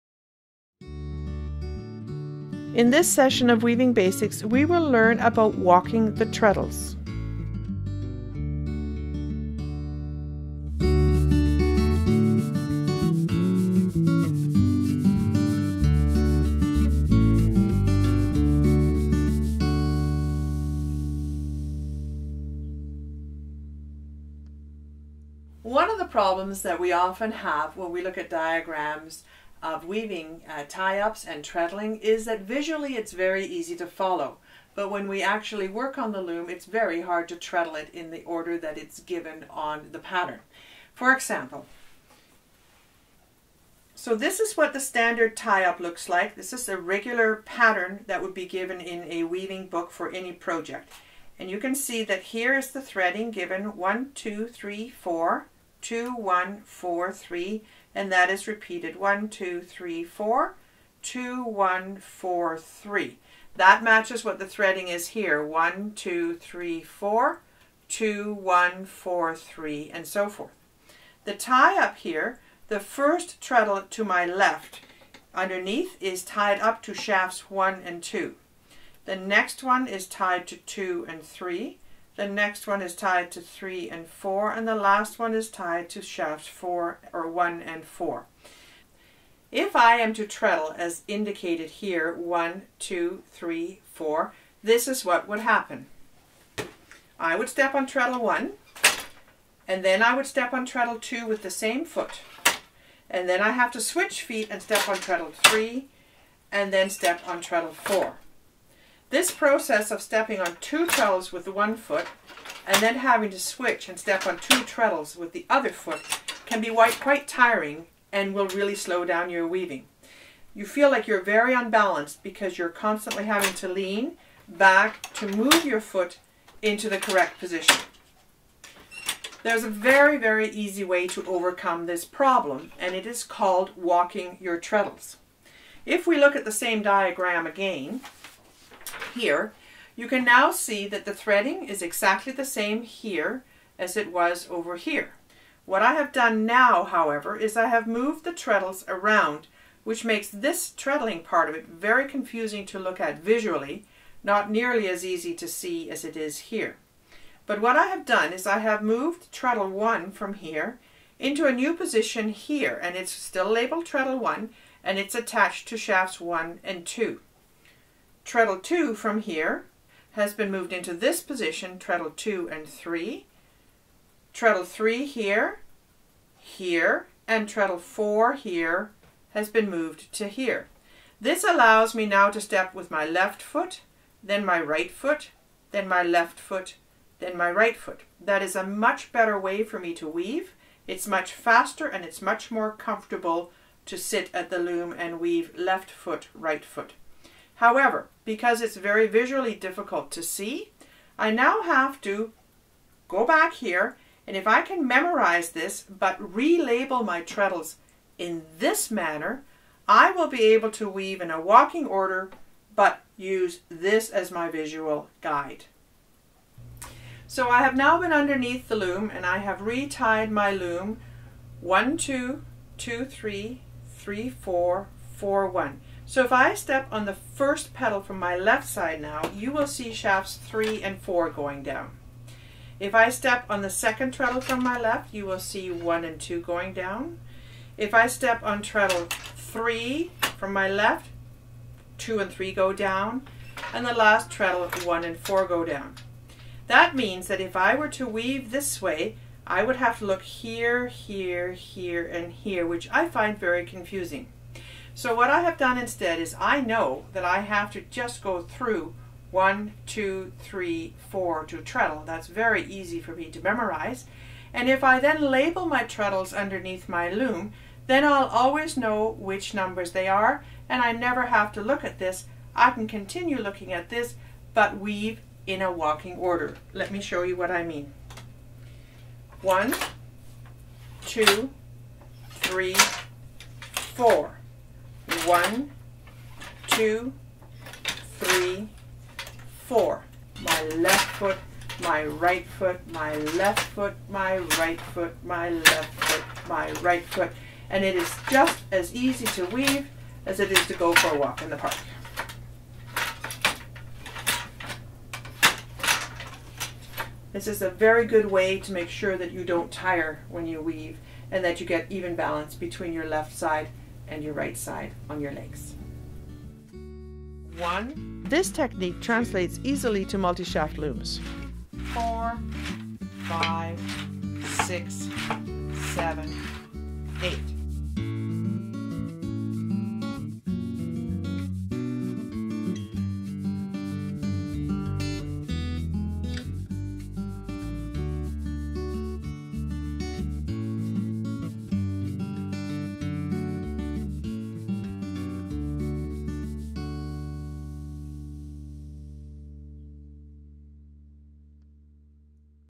Weaving Basics: Walking the Treadles
In this session of Weaving Basics, we will learn about walking the treadles.

Instruction by Jette Vandermeiden from Teanaustaye Textiles Weaving Studio

Filmed and edited by David Killing